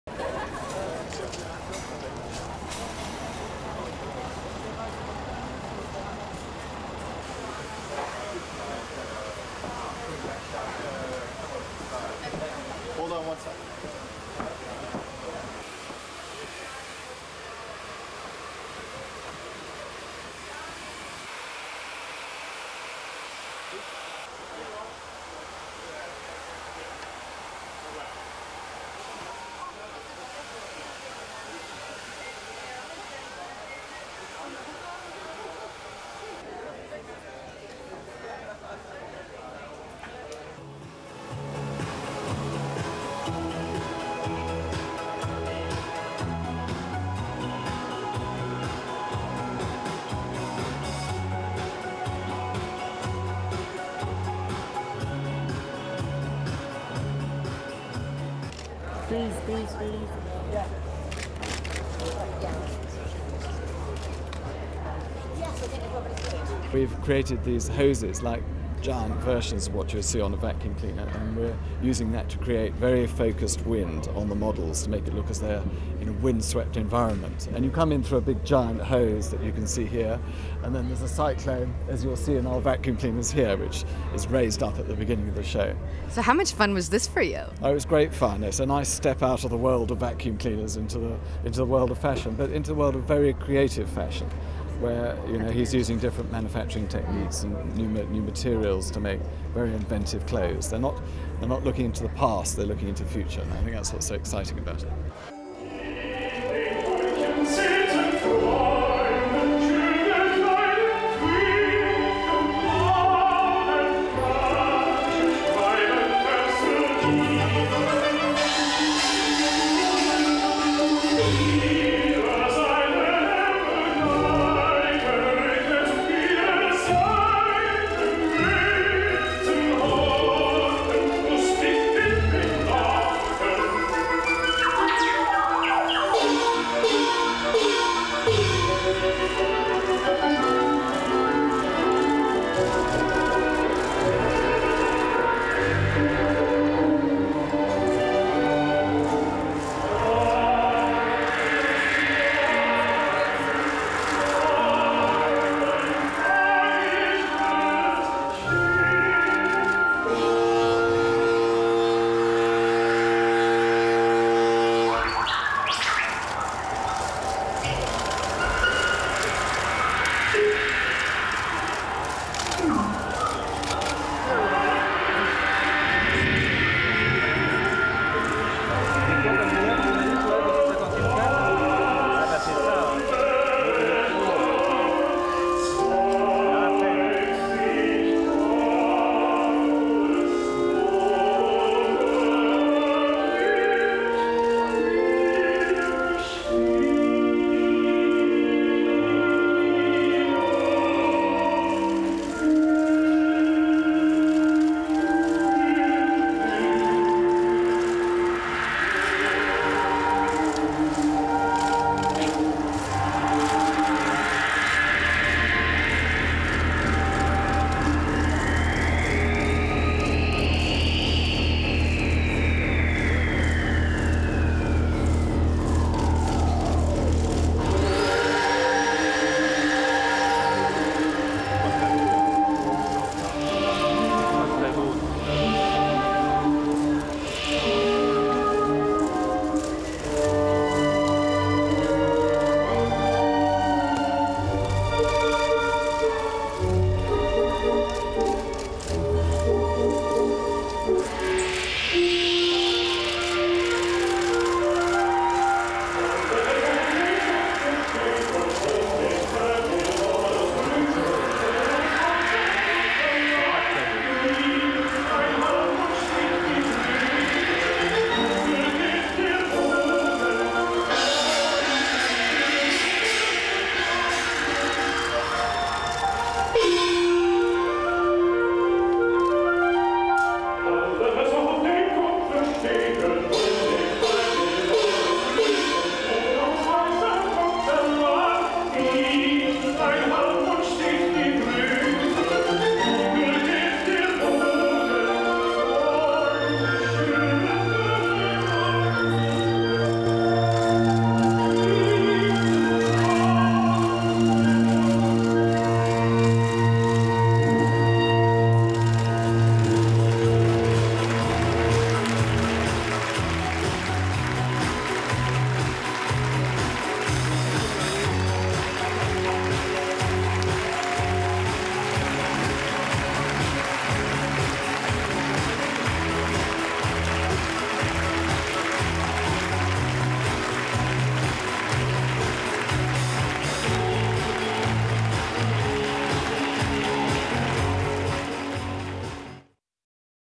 Japanese designer brings vacuum cleaners to the catwalk
AP Entertainment
Paris, 2nd October 2007
1. Tilt up exterior
2. Various shots backstage preparations
3. Pan to rehearsal
4. Various of Dai Fujiwara
5. Wide shot rehearsal
6. Medium shot James Dyson
7. SOUNDBITE (English) James Dyson:
"We've created these hoses, giant versions of what you get on the vacuum cleaner and we are using that to get very focussed wind on the models to make it look as though they are in a very windswept environment. And you come in through a bit giant hose which you can see here and then there is psychlone as you can see in our vacuum cleaners here, but raised up at the beginning of the show."
("Q: How much fun was this for you?")
"It was great fun, it's a nice step out of the world of vacuum cleaners in to the world of fashion. In to the world of very creative fashion where's using different manufacturing techniques and new materials to make very inventive clothes. They are not looking in to the past they are looking to the future and think that's what's so exciting about it."
8. Highlights from catwalk show
9. Dai Fujiwara and James Dyson on catwalk
Dai Fujiwara became the first fashion designer to draw inspiration from a vacuum cleaner, collaborating with British inventor James Dyson to produce a whirlwind display of gently wafting dresses and billowing coats at the Paris ready-to-wear shows Tuesday (02OCT07).
The creative director of the Japanese label Issey Miyake enlisted Dyson to produce a set featuring huge yellow tubes that sent gusts of air swirling through models' hair and clothes in a poetic reflection on global climate change.
Men and women paraded through a tent next to the Louvre Museum in lustrous pleated outfits that bounced with every step, while diaphanous nylon jackets printed with coloured confetti blew up like parachutes in the back.
Dai Fujiwara has been a member of Miyake's team since 1994, helping to develop A-POC (an acronym for "a piece of cloth"), a breakthrough manufacturing process that allows clothes to be woven in a single piece with no sewing involved.
For this collection, he took apart Dyson vacuum cleaners and incorporated their components into his designs, adding a circular pocket to a coat or wrapping a tubular belt around a dress.
The collaboration is the latest example of designers striking alliances with large industrial firms.
Giorgio Armani announced recently he would design a flat-screen television for South Korean electronics maker Samsung, while fellow Italian house Versace is developing interiors for helicopters and private jets.